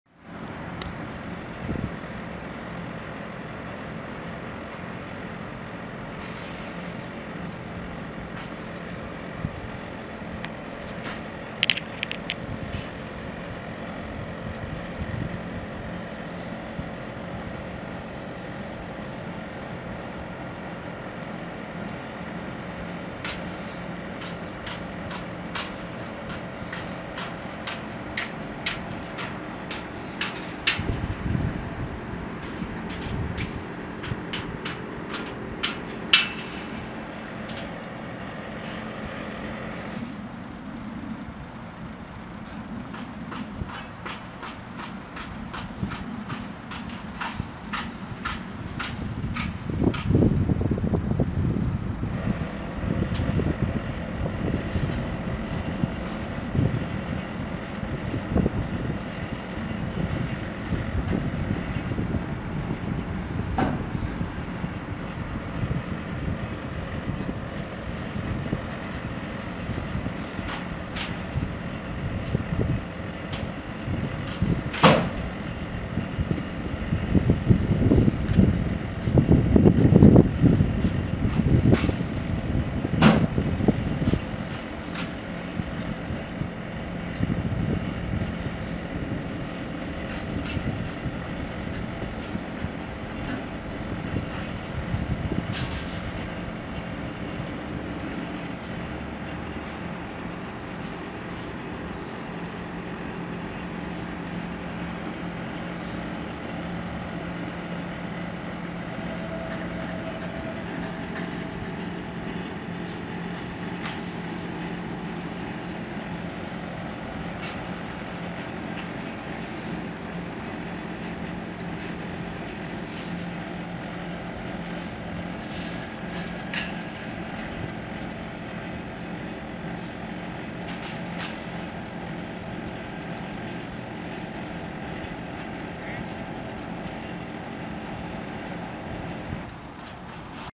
Bonfire Fun Fair Pull On, Goose Fair Site, Nottingham - 1st November 2017
Use this link to view Nottingham Goose Fair listings on eBay